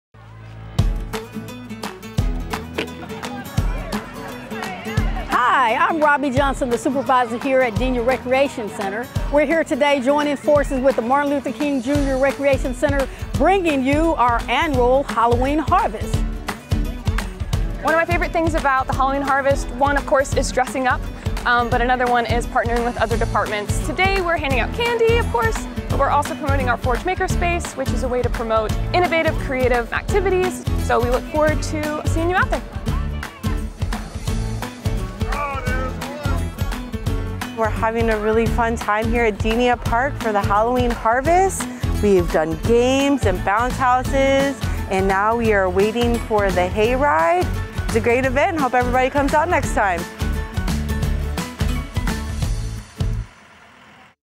Denton Halloween Harvest 2023
Denton Parks & Recreation hosted its annual Halloween Harvest at the Denia Recreation Center and we had so much fun! There were plenty of special activities, candy, and even a hayride!